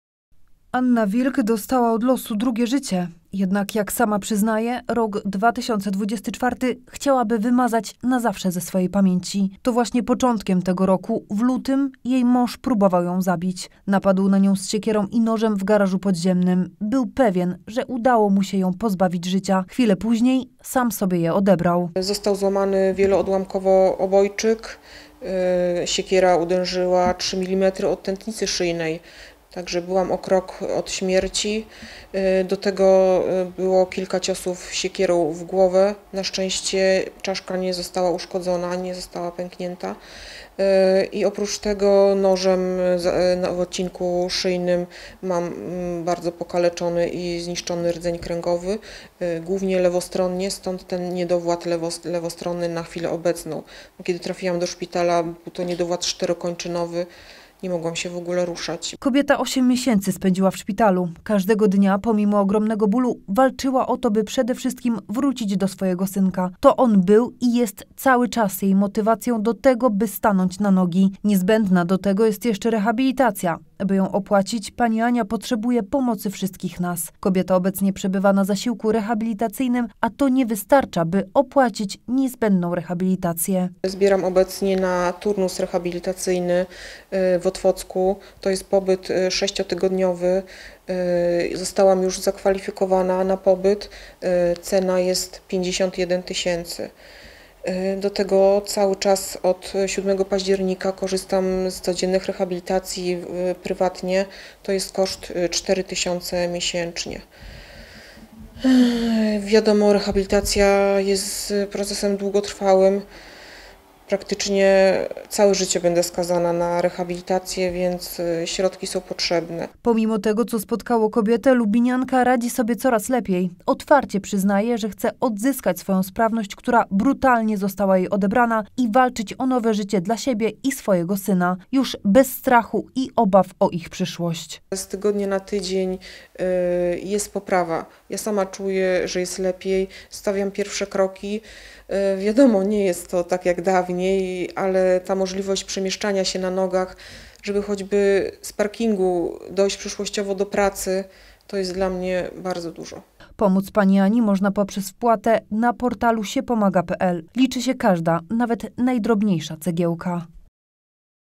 Lubin Walczy o życie po ataku męża
Jej mąż próbował ją zabić. Nie udało mu się, jednak odebrał jej tak cenne zdrowie. Anna Wilk spędziła w szpitalu osiem miesięcy każdego dnia walcząc o swój powrót do zdrowia. W dalszej walce o swoją przyszłość lubinianka potrzebuje naszej pomocy.